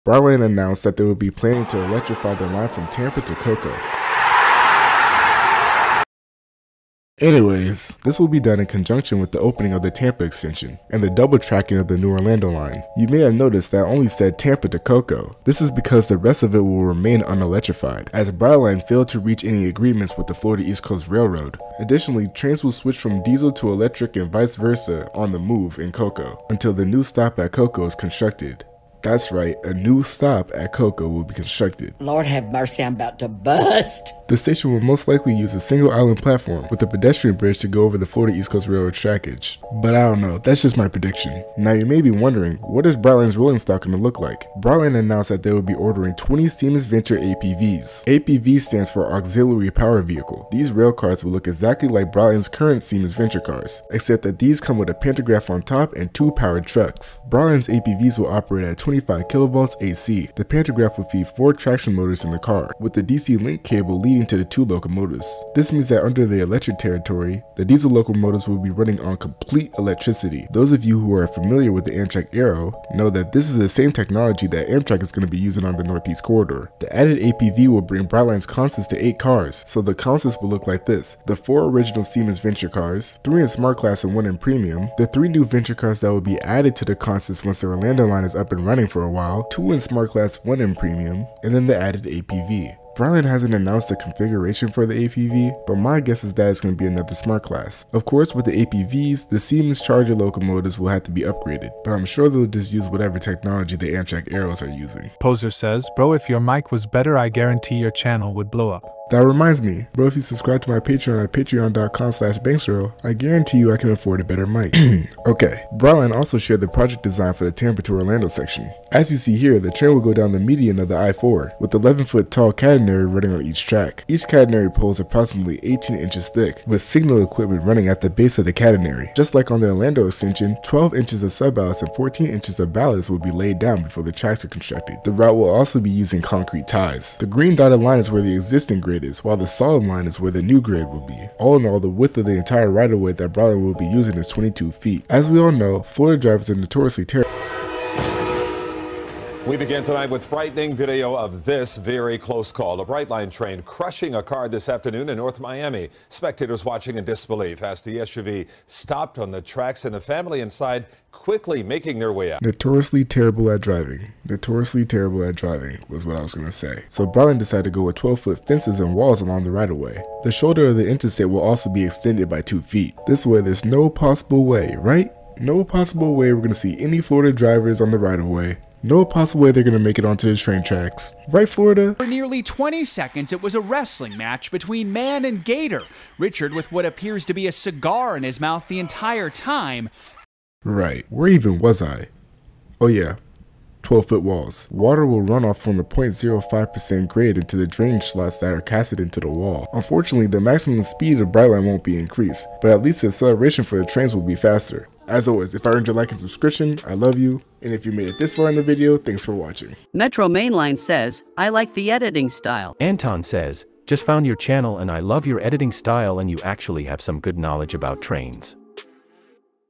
What a Brightline Electrification Would Look Like...
Brightline FINALLY Announces Electrification Plans for Florida with new Siemens Venture Cars. The Electrification will be between Tampa and Cocoa.

This is the only way I know that people are really interested in the topic. Plus it would mean a lot.